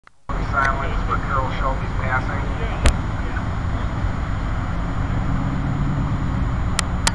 SAACMCR2012SHOW0012.AVI
Shelby American Automobile Club - Motor City Region 2012 car show.